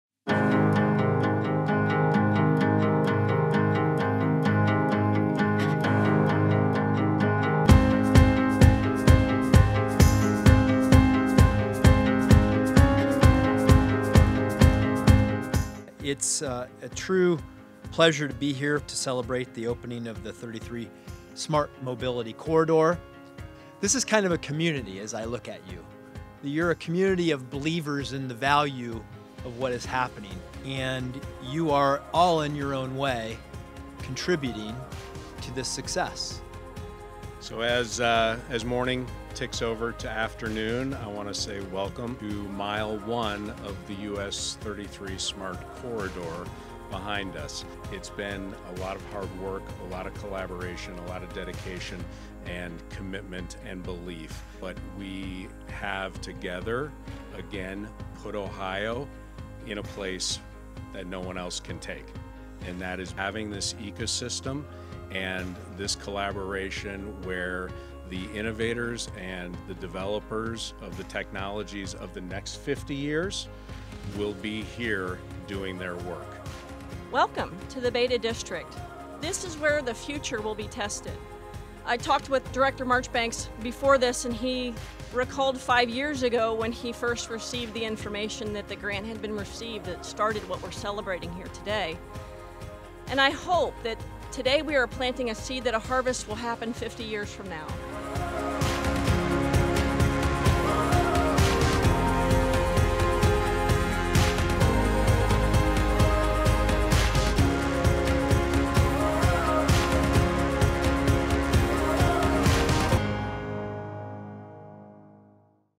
33 Smart Mobility Corridor
Dublin joined partners from around the state and region to launch the world’s most connected highway. The 33 Smart Mobility Corridor, a living lab of The Beta District, runs from the City of Dublin on the east end through the City of Marysville, past industry partner Honda’s manufacturing and vehicle development operations, and concludes at the gates of the Transportation Research Center (TRC) Inc. in East Liberty, Ohio.